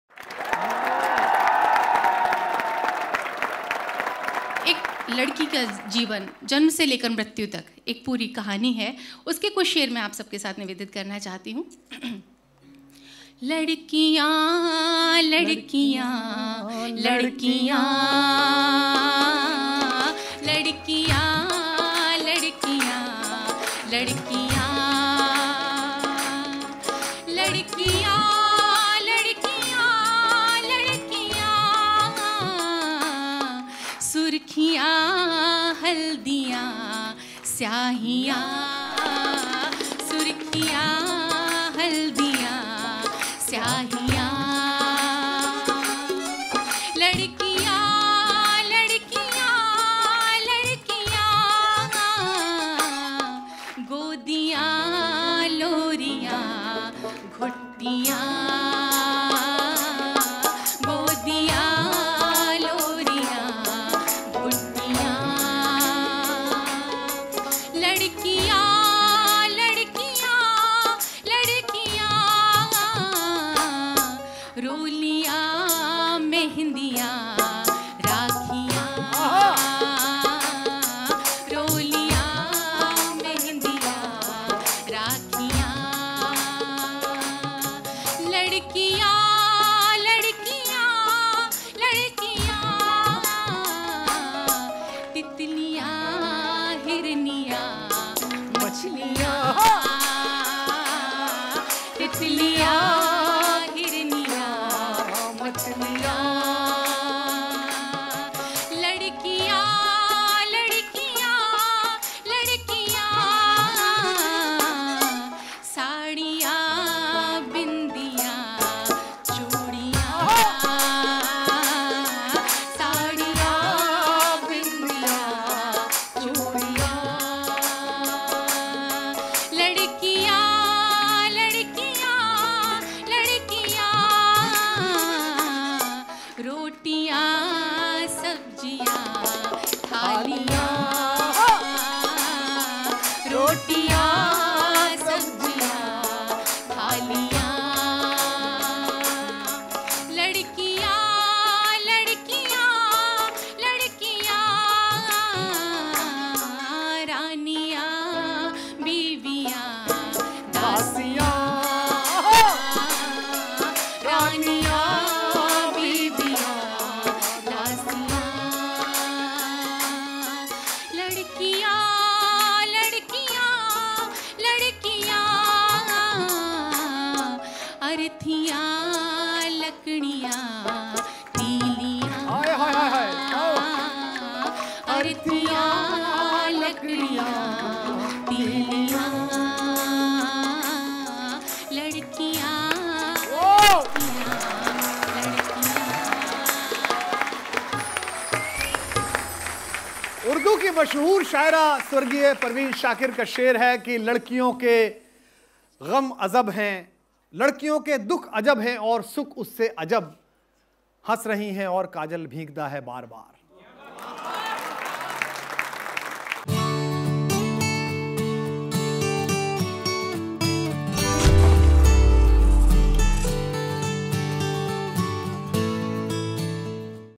Kumar Vishwas | लड़कियों के जीवन को बयां करती कविता | Sonroopa Vishal | Sahitya Tak
कवि कुमार विश्वास के साथ आज तक की ओर से आयोजित कवि सम्मेलन में कवयित्री सोनरूपा विशाल ने मंच साझा किया.  उन्होंने लड़कियों के जीवन को बयां करने वाली एक शानदार कविता सुनाई. साहित्य तक पर सुनिए यह कविता.